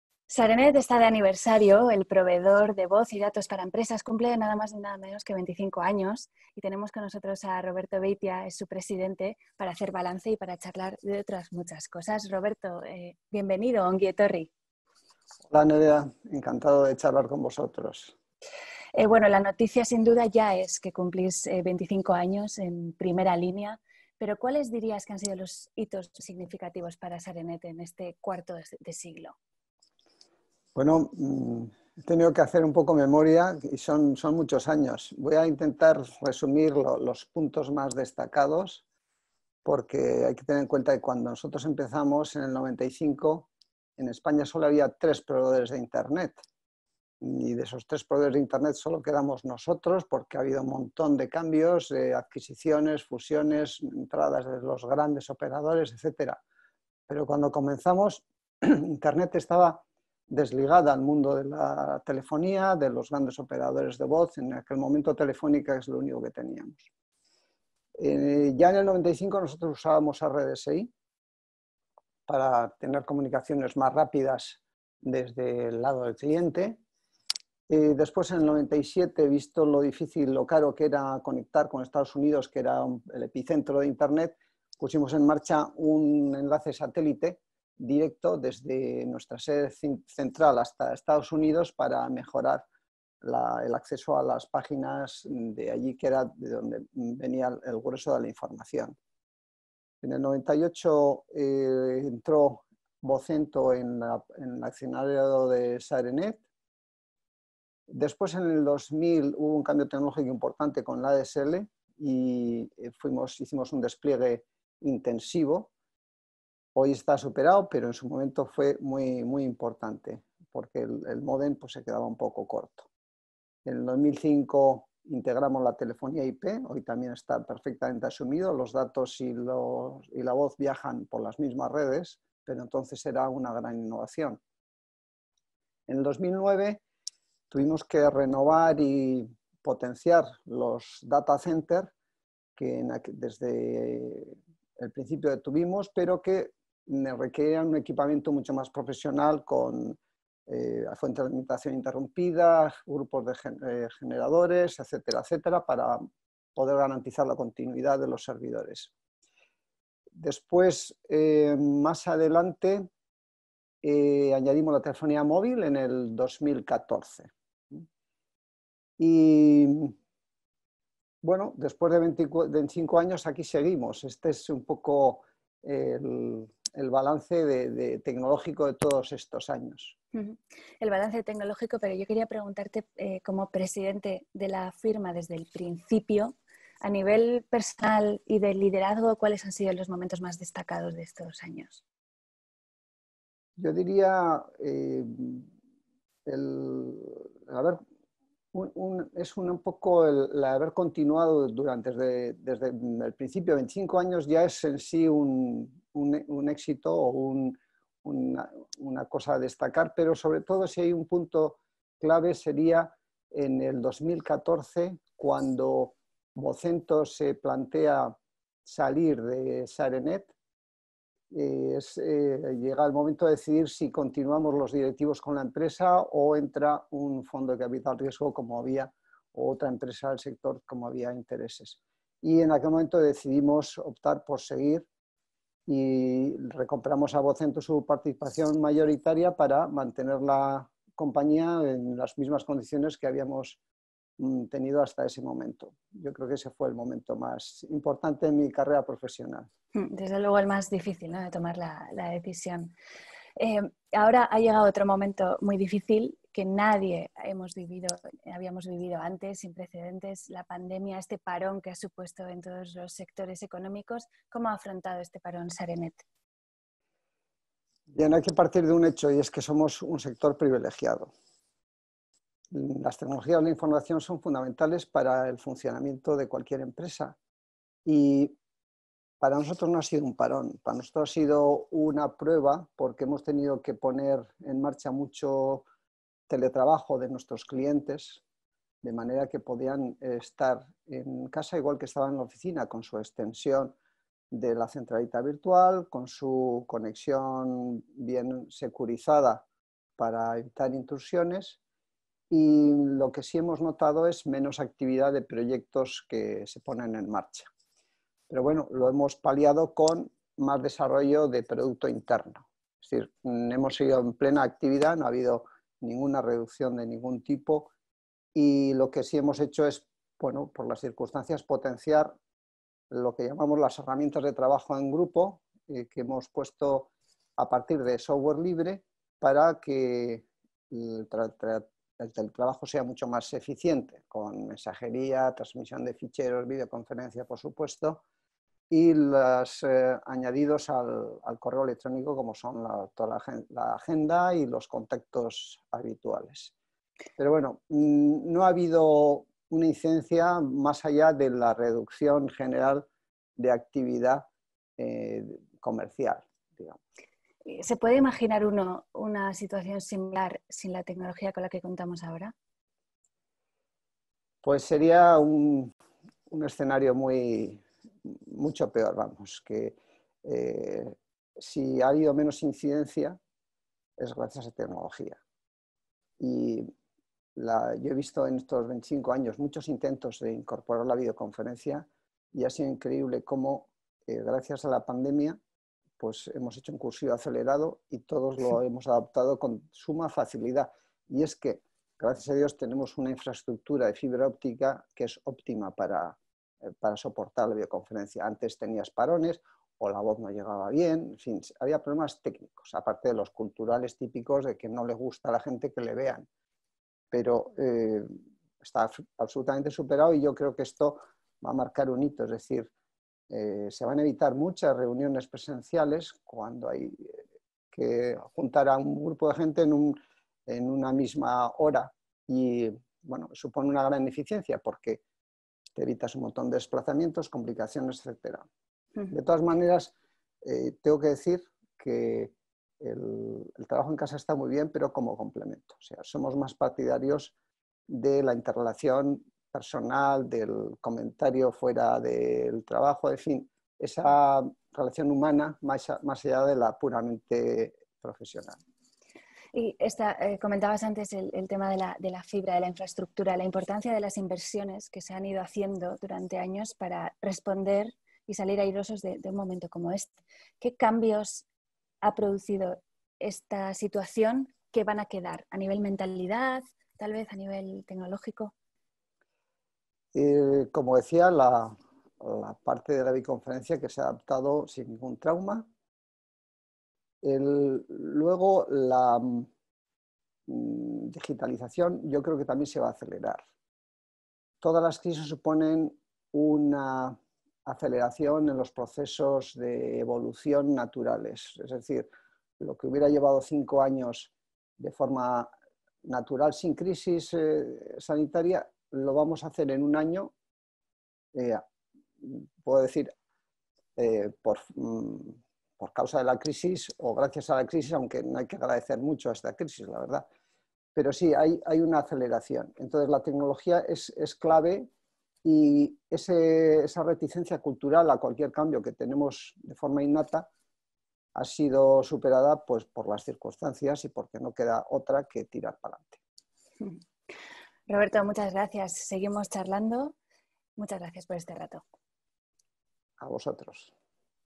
Entrevista a Roberto Beitia, presidente de Sarenet, en Computer World
Entrevista de Computer World al presidente de Sarenet, Roberto Beitia.